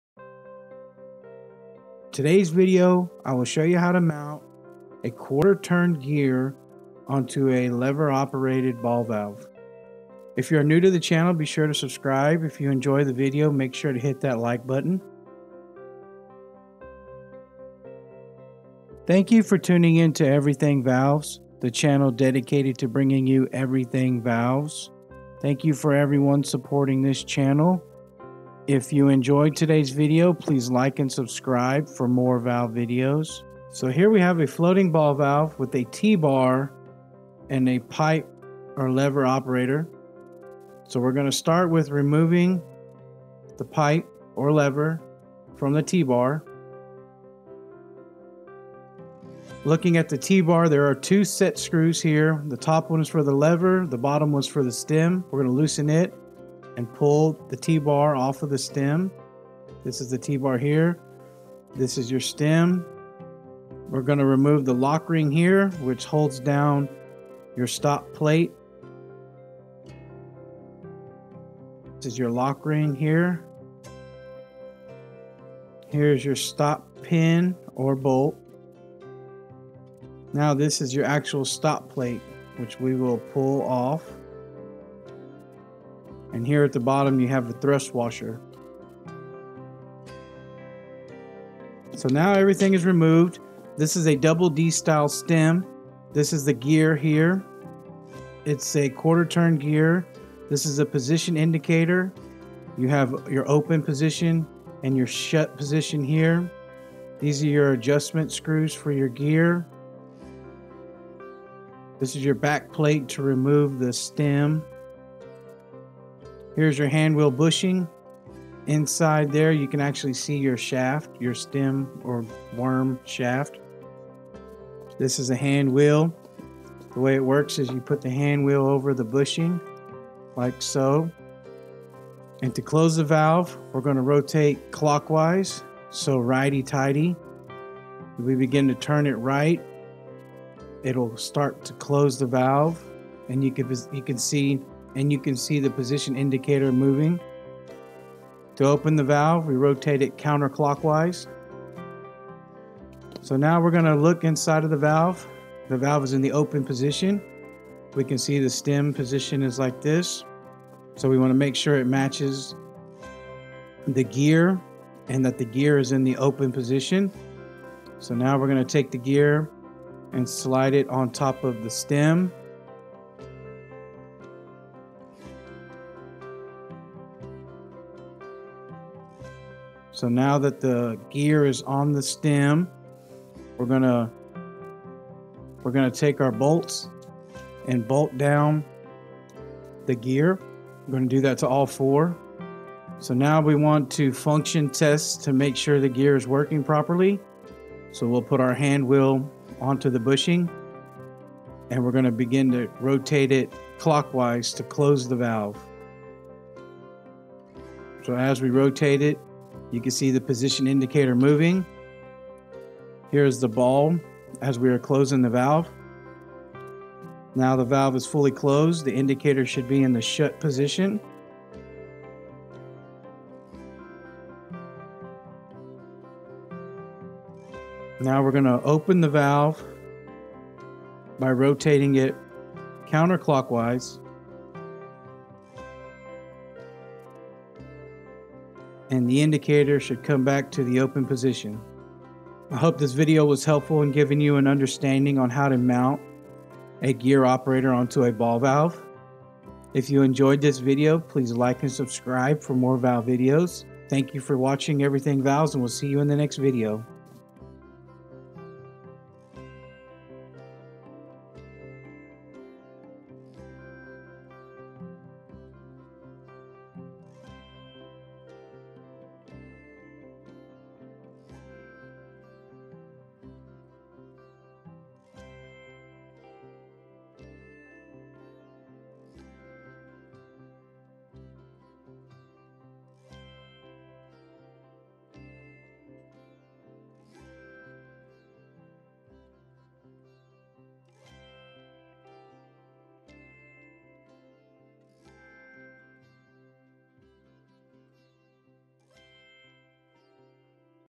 How to install a quarter turn gear onto a ball valve. Floating ball valve repair. Ball Valve how to
Welcome back to everything valves. The channel dedicated to bringing you everything valves.

This video will give you a good idea of how the stem and gear function on a ball valve. This is an 8" Class 150 floating ball valve. It originally was lever operated. "When I say lever it really is a pipe". The T bar is actually what is fixed to the stem. When the pipe is inserted into the T bar you then would rotate clockwise to close and counter clockwise to open.

Removing all of the T bar components is pretty easy as you can see. The actual gear is what can get complicated. This video I kept it very simple. The overview of the gear covers the main components. Anything else would actually require us opening the gear. "Gear video coming soon". The key when installing the gear is to make sure the valve is open and the gear is also in the open position. If not you can potentially install the gear incorrectly.

I hope you are enjoying these valve how to videos. Please leave a comment on what you thought or what you would like to see. I appreciate your support.